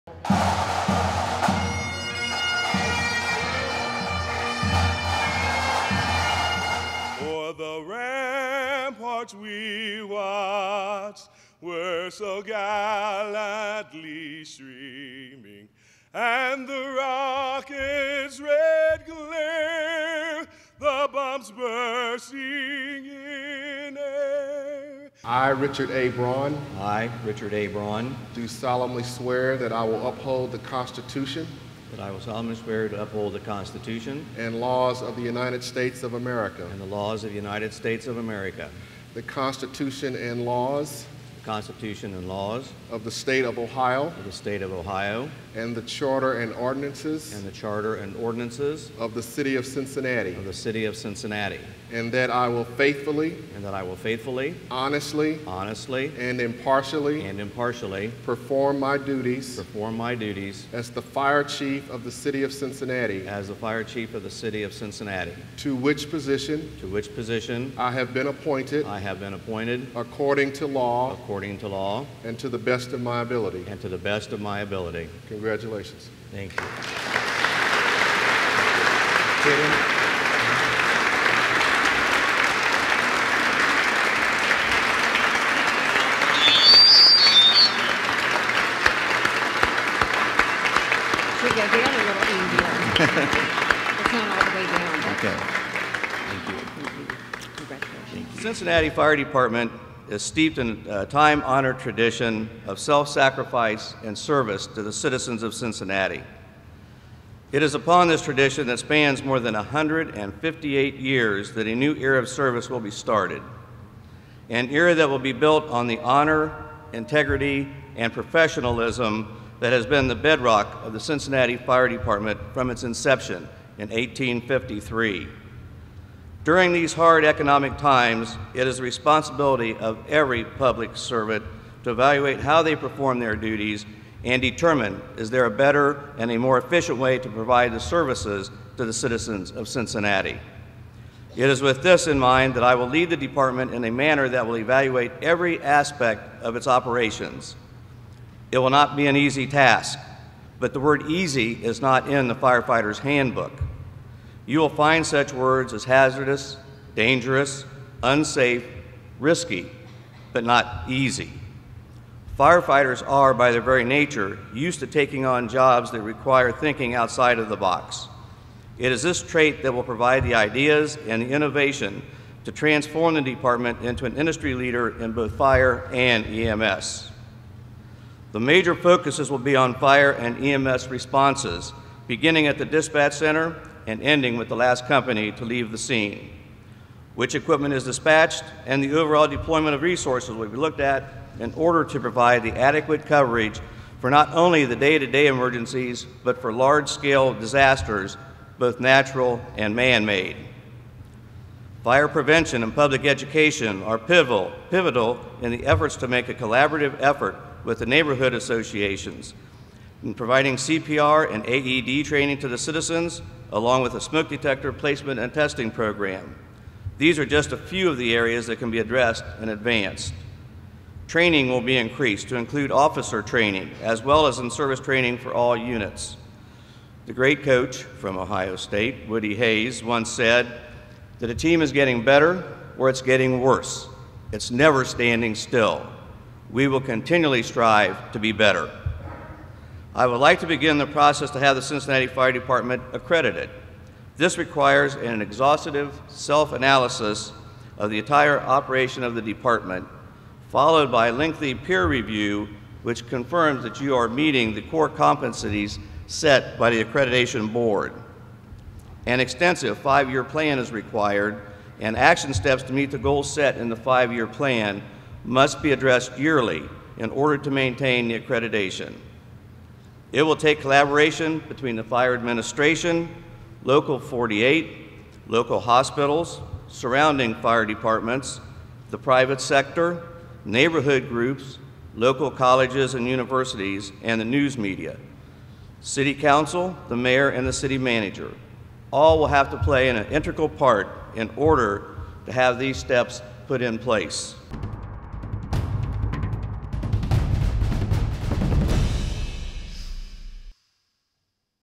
New Fire Chief is Sworn In
Richard A. Braun takes the oath of office as Cincinnati's new fire chief at Station 51 in College Hill Tuesday morning.